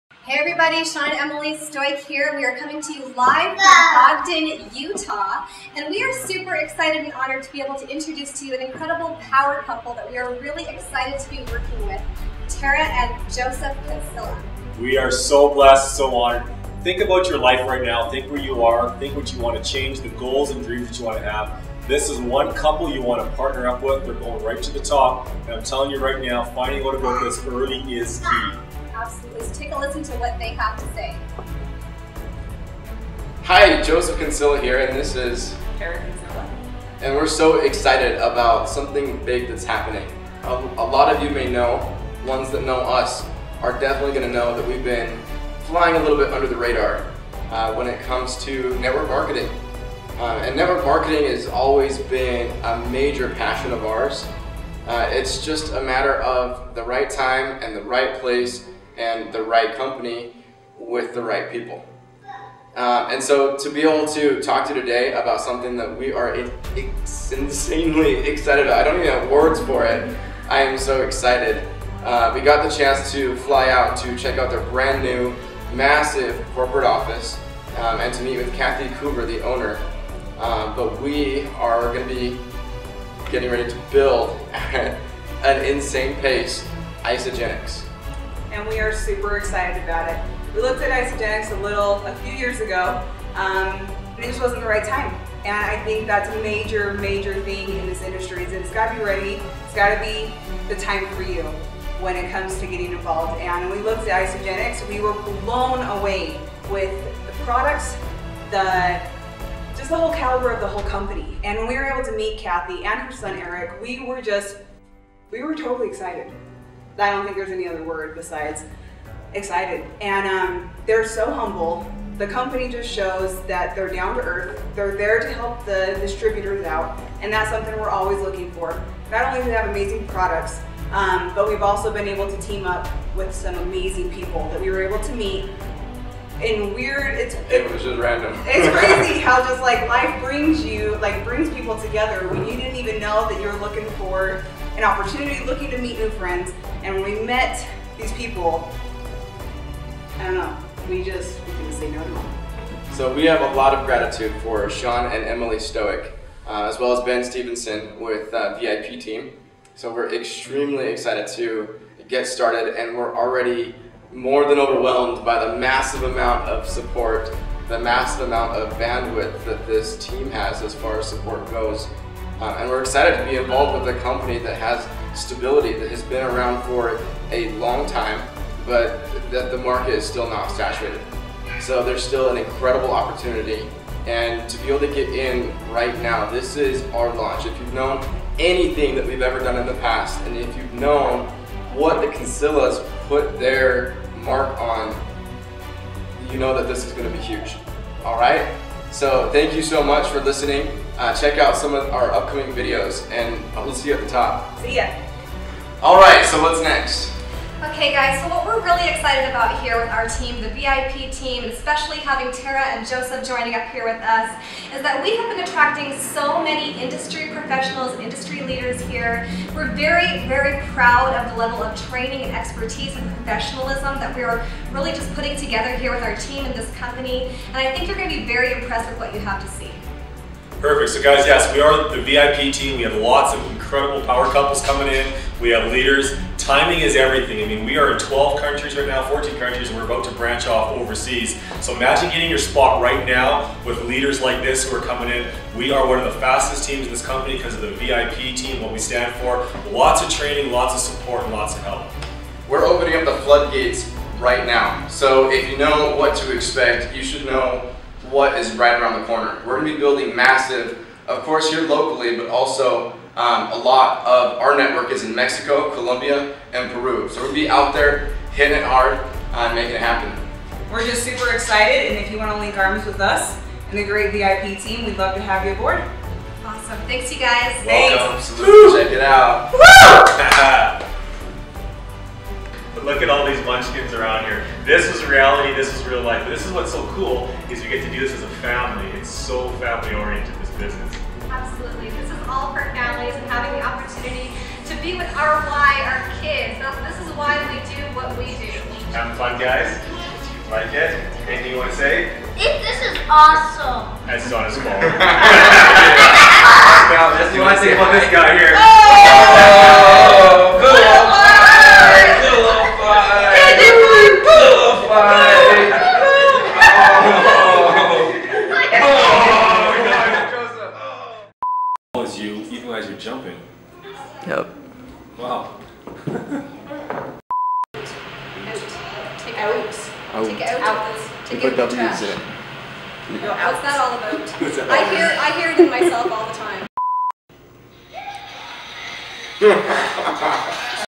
Joseph and Tara Cancilla VIP Team LAUNCH!
Joseph and Tara Cancilla Power Couple Home Business Leaders ViP Team Launch Announcement with VIP Team Leaders Shawn and Emily Stoik!!
What are Joseph and Tara Cancilla doing with the #1 Home Business Team?  You won't want to miss teaming up with this Power Couple who are tremendous leaders with servants hearts!